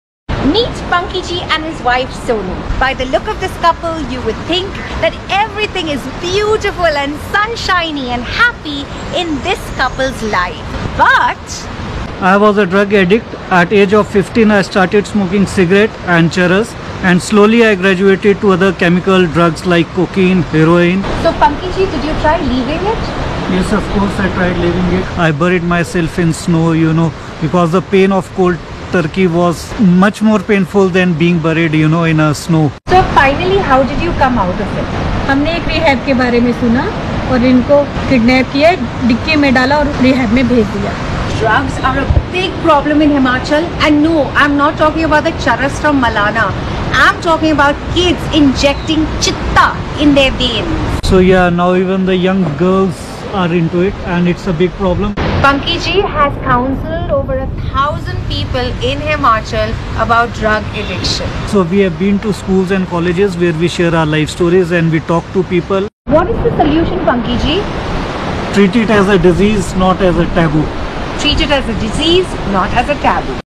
Shehnaz Treasury in Sunshine Tirthan Vallley
Shehnaz India's biggest travel influencer gives a message about drugs with Panki Sood  staying at his beautiful riverside property !